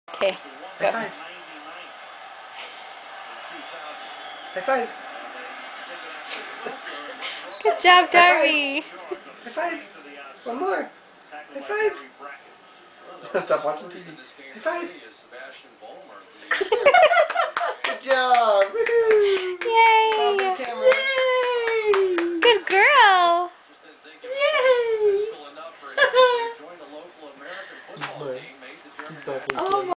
Darbi giving high-5's
Darbi gives Dad high-5's!